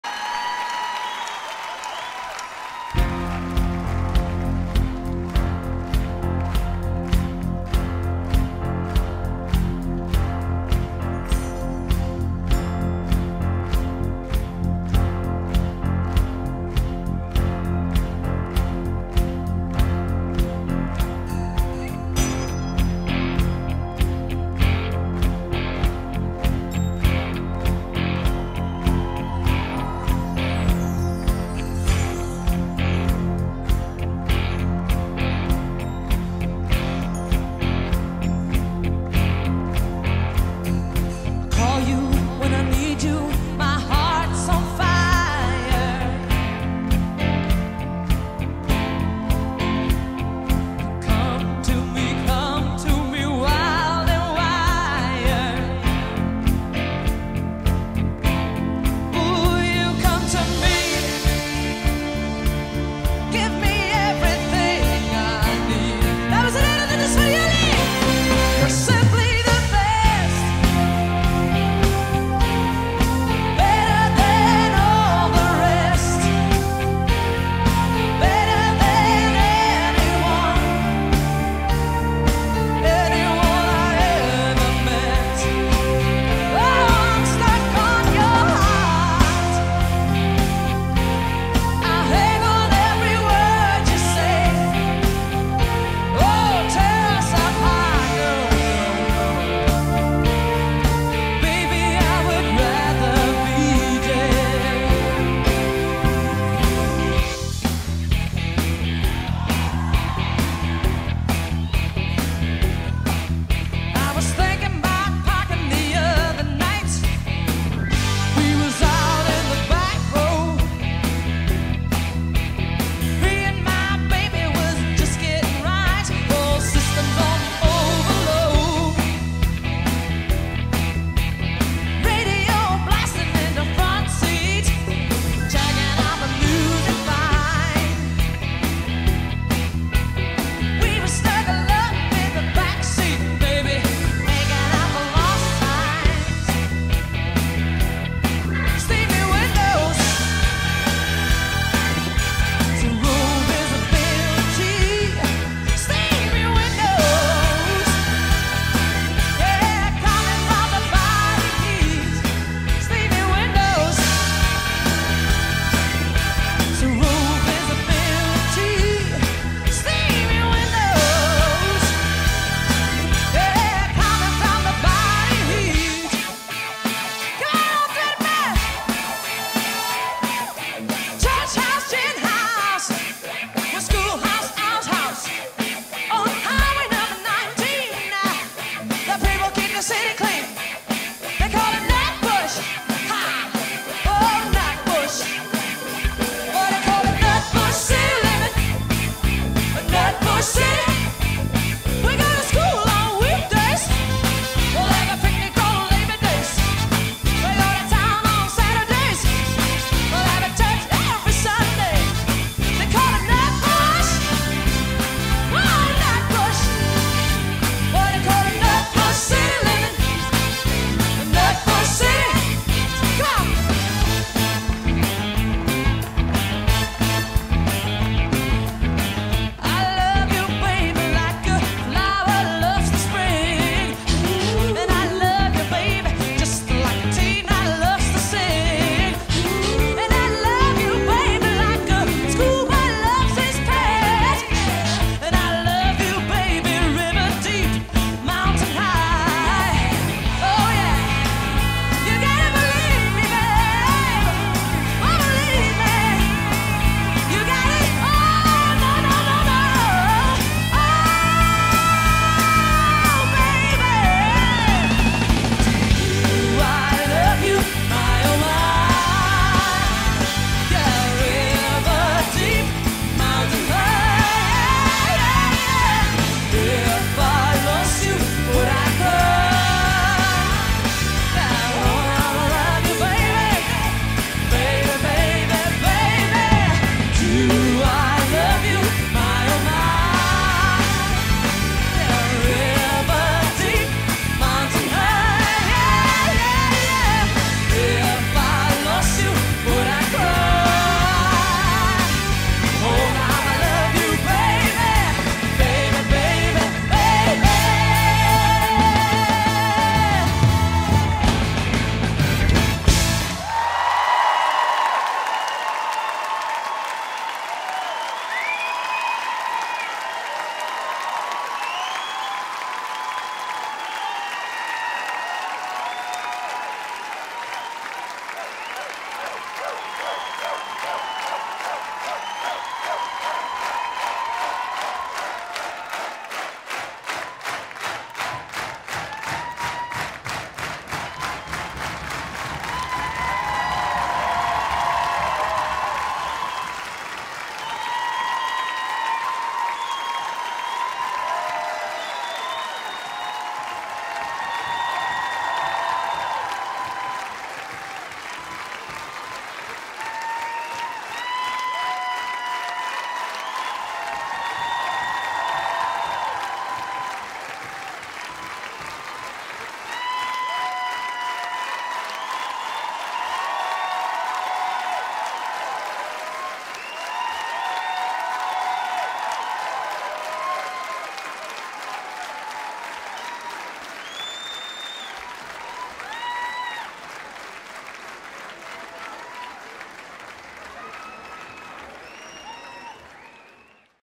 Natalia - Tina Turner Medley - Back For More Tour 2004 - Zuiderkroon Antwerpen
Natalia brengt een Tina Turner Medley tijdens haar Back For More Tour in 2004.